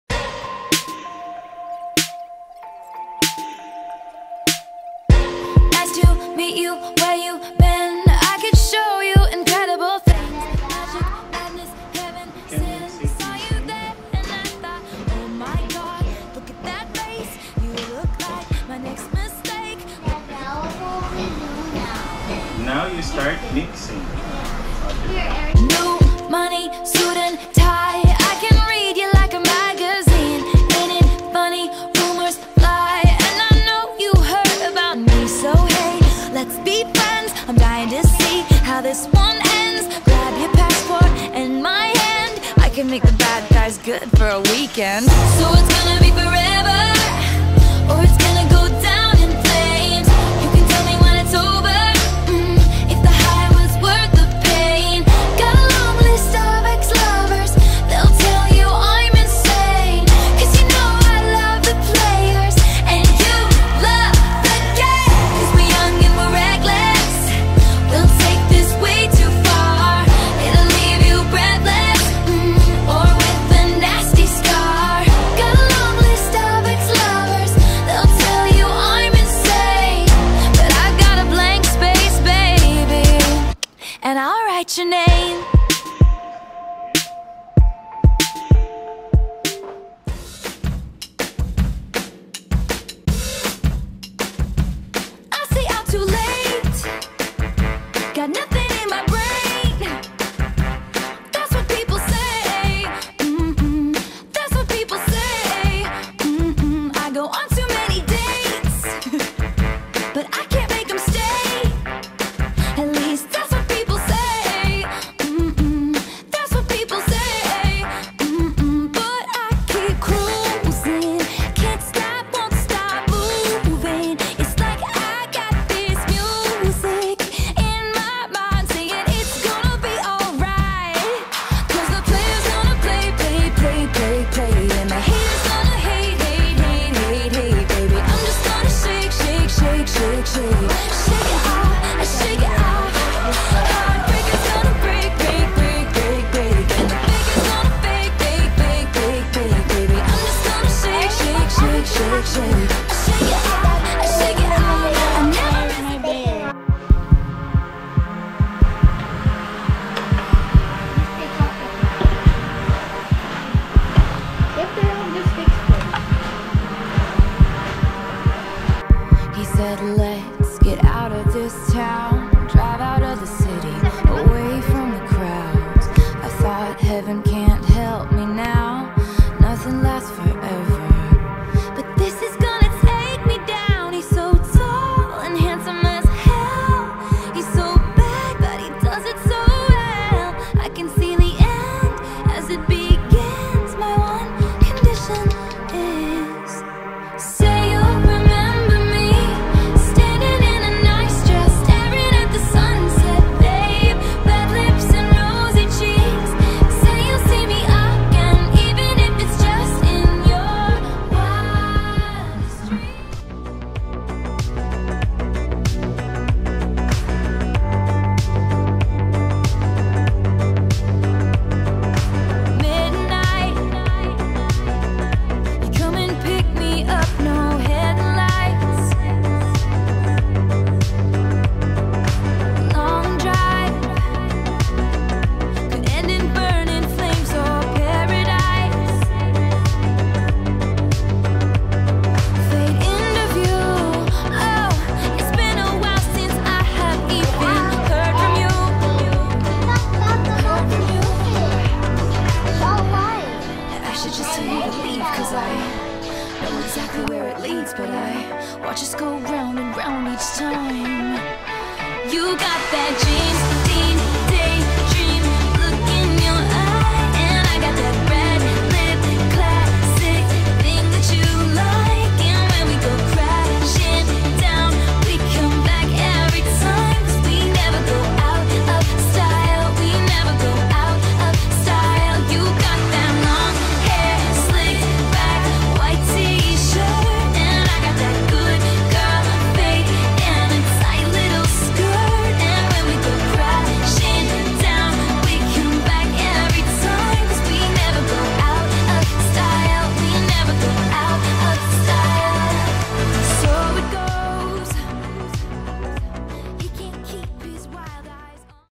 The Magnet Activity
The Magundayao cousins made refrigerator magnets during this 2022 summer vacation.